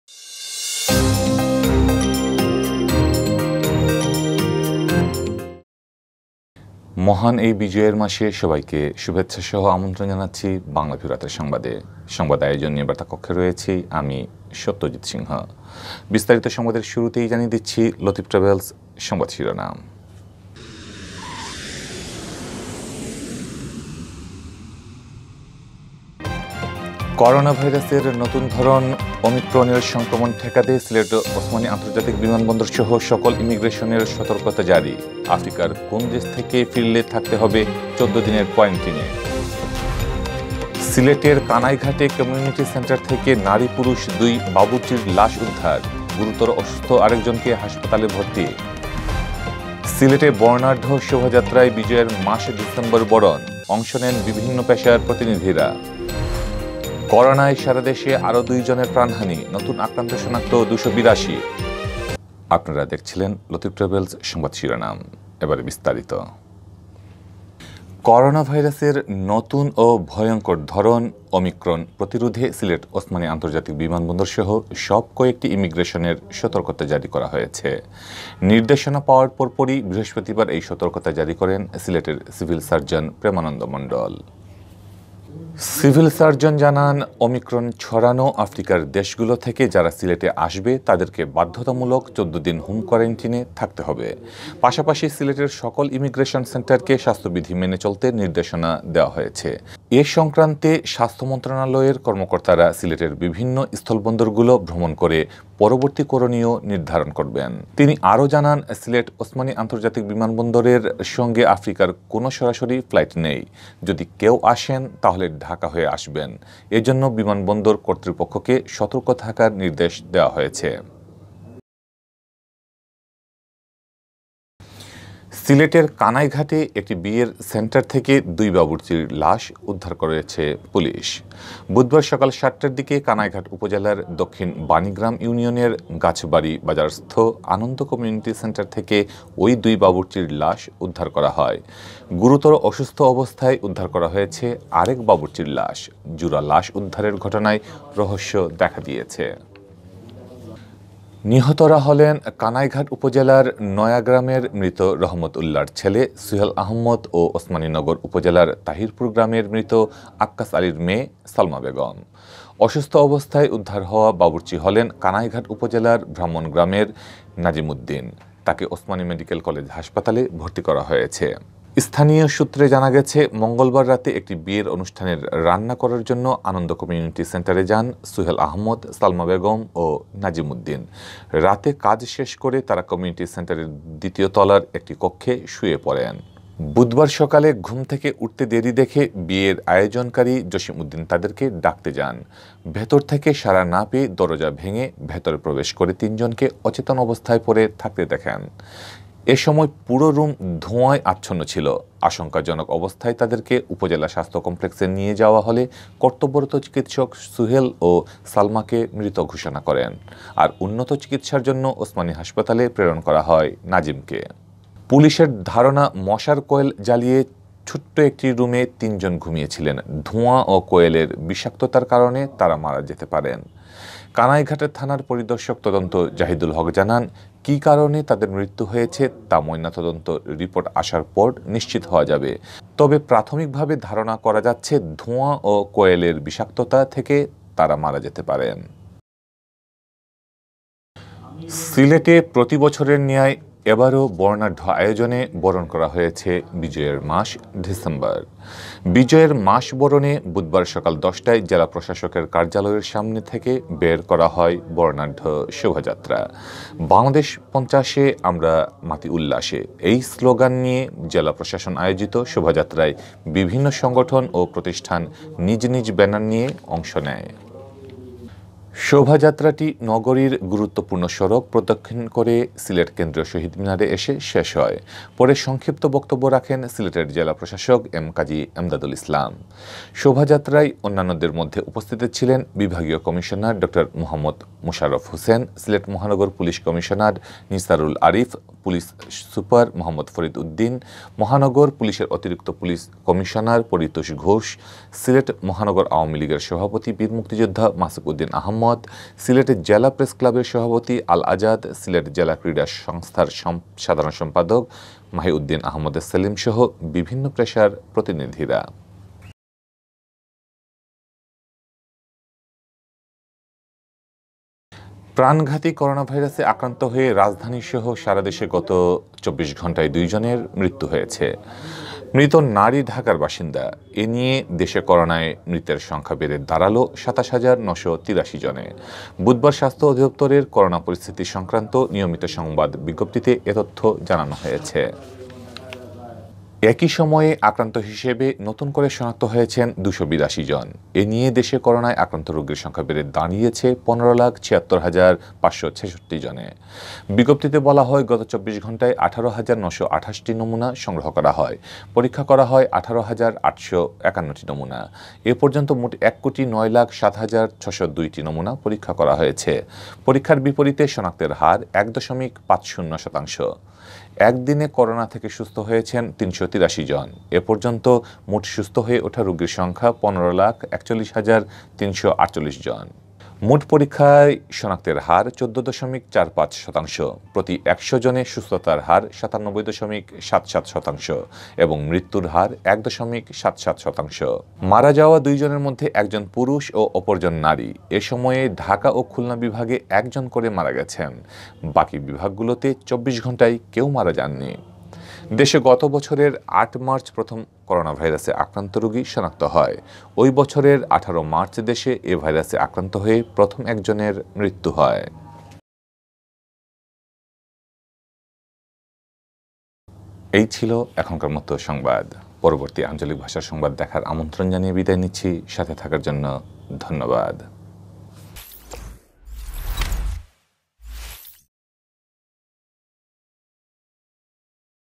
বাংলাভিউ সংবাদ(০১,১২,২০২১)।। Banglaviewtv // Banglanews 01.12 . 2021
করোনাভাইরাসের নতুন ধরন ওমিক্রনের সংক্রমণ ঠেকাতে সিলেট ওসমানী আন্তর্জাতিক বিমানবন্দরসহ সকল ইমিগ্রেশনের সতর্কতা জারি। আফ্রিকার কোন দেশ থেকে ফিরলে থাকতে হবে ১৪ দিনের কোয়ারেন্টিনে।

সিলেটের কানাইঘাটে কমিউনিটি সেন্টার থেকে নারী-পুরুষ দুই বাবুর্চির লাশ উদ্ধার। গুরুতর অসুস্থ আরেকজনকে হাসপাতালে ভর্তি।